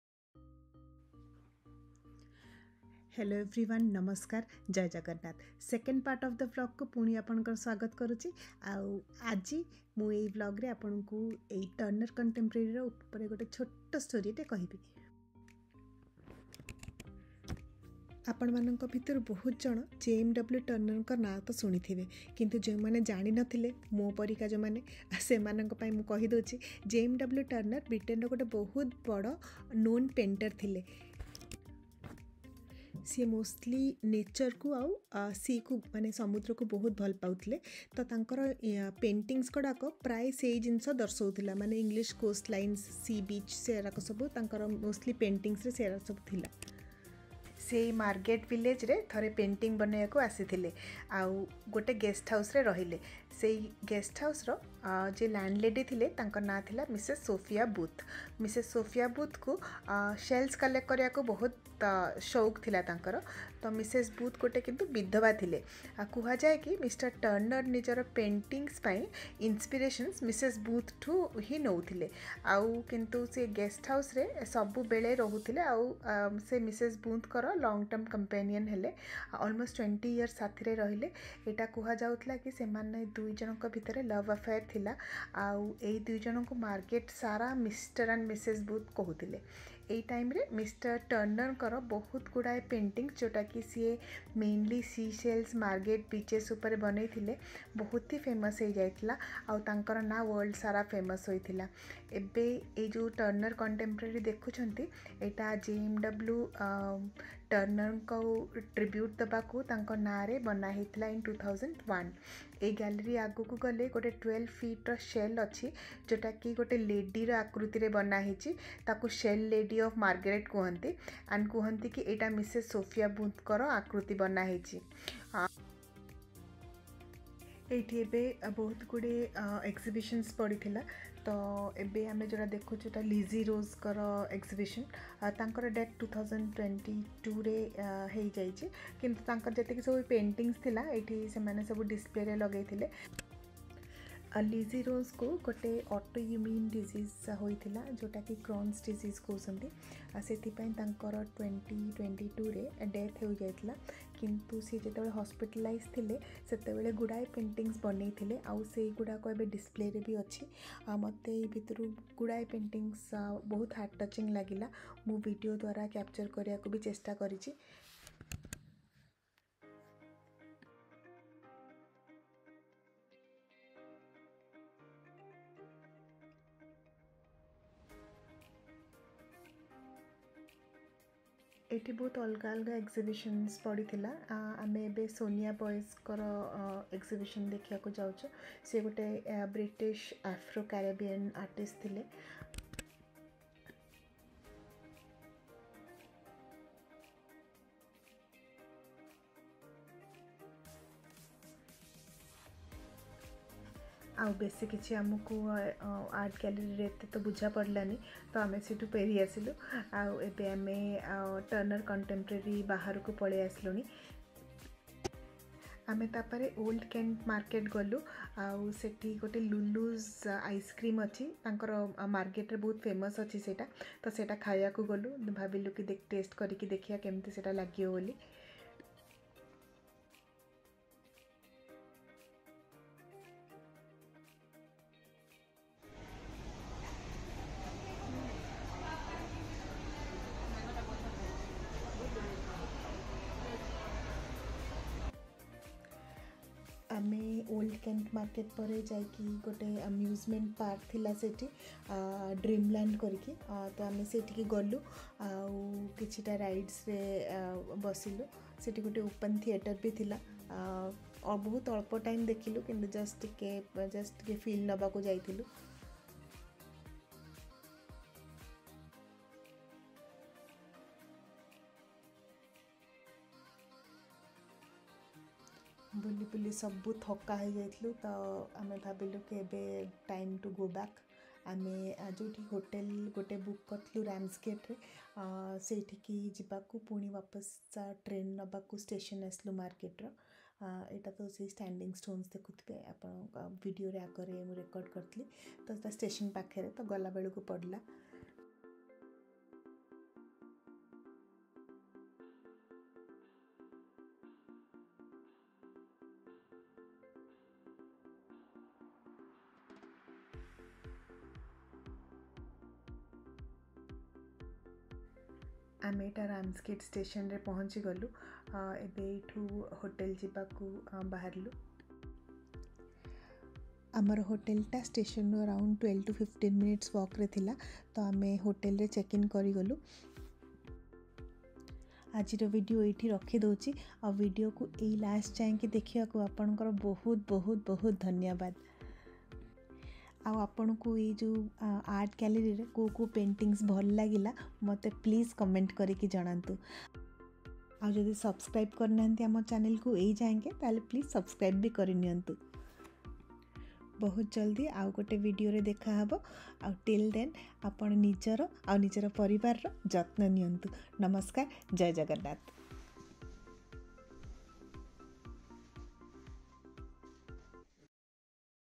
Exploring the Turner Contemporary Museum , Margate UK|Expressive Paintings|Which one did you Like?
Turner Contemporary is a renowned art gallery located in the picturesque coastal town of Margate, situated in southeast England. The gallery is named after the famous British landscape painter J.M.W. Turner, who was known for his innovative use of light and color in his artwork. Just like Turner's revolutionary approach to art, Turner Contemporary aims to inspire and challenge visitors with its cutting-edge contemporary art exhibitions.